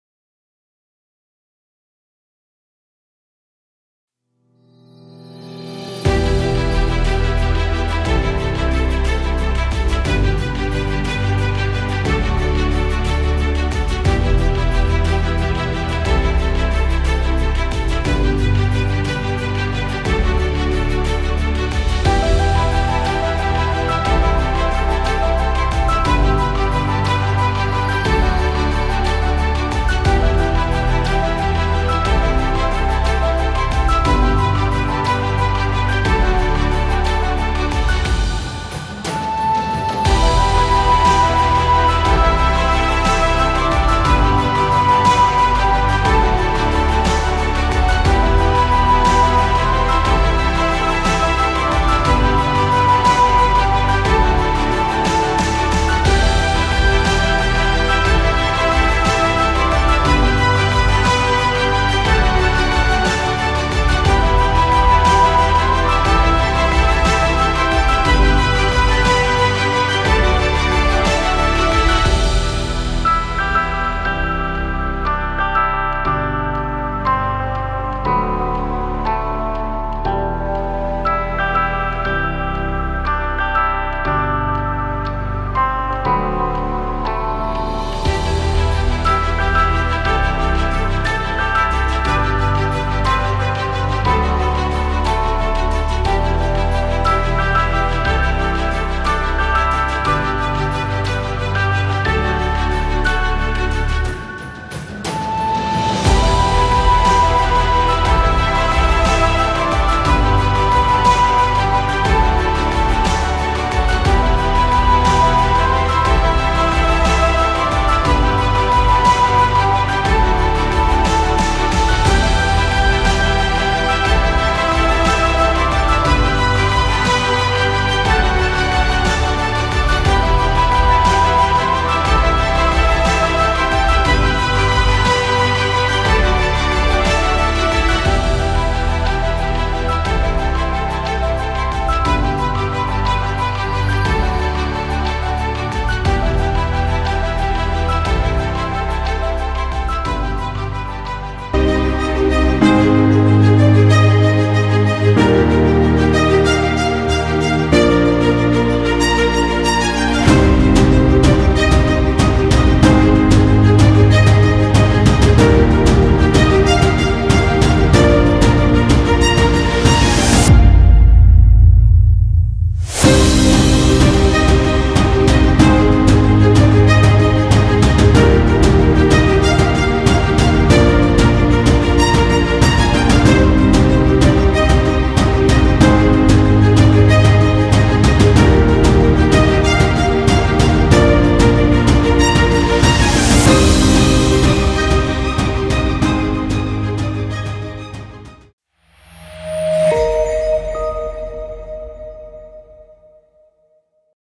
You Won't Believe What YAHUAH Just Revealed
when I was digging deep into the Day of YAHUAH for my last video, I stumbled upon some encouraging scripture for Yasharel!